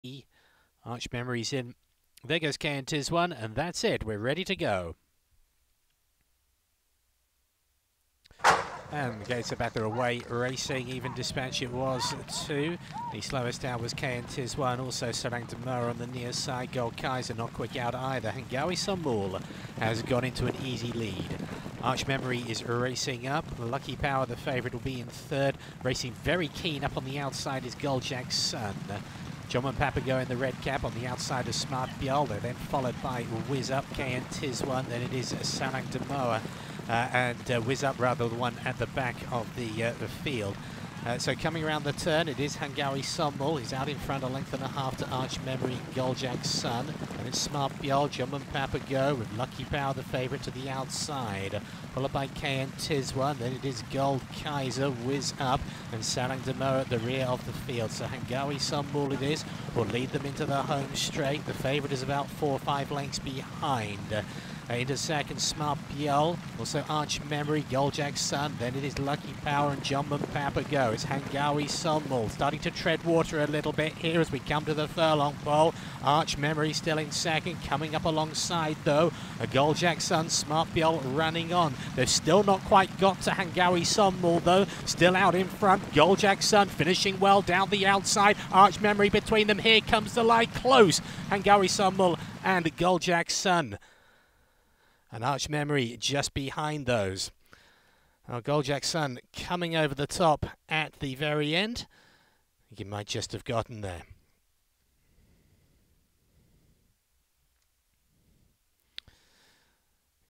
(Seoul) 2023.04.23 R4 Class 5 (1300M) Handicap
W: Guljak Sun (KOR)
J: Lee Hyeok
T: Park Youn-gu
O: Park Deok-hui